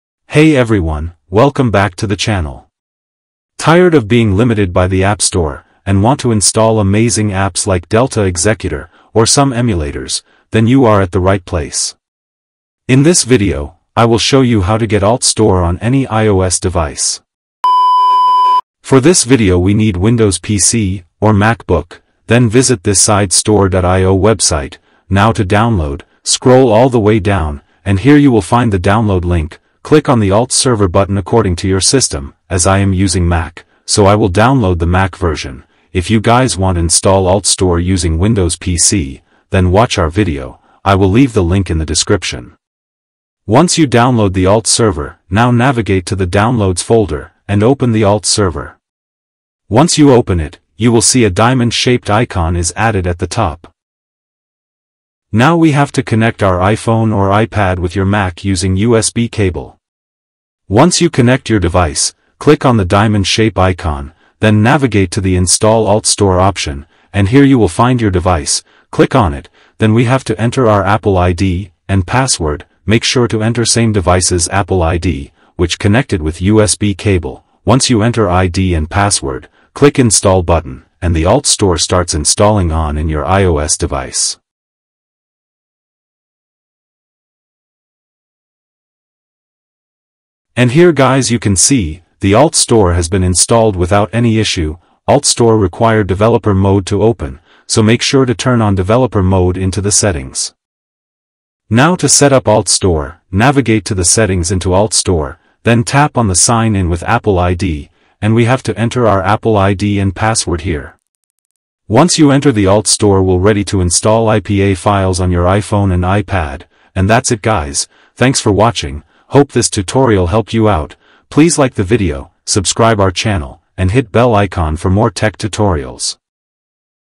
iOS 26: How To Install AltStore On iPhone & iPad (2026) | AltStore iOS Guide
In this video we will learn how to download and install AltStore On iOS Device.

AltStore Guide
AltStore Tutorial
Install IPA Files
AltServer Guide

Time Stamp
0:00 Intro
0:19 Download AltServer
0:43 Install AltStore
1:35 Setup AltStore
1:53 Outro

Installing third-party apps or using AltStore may void your warranty or breach Apple's policies. Proceed at your own risk. I am not responsible for any issues that may arise from following this tutorial.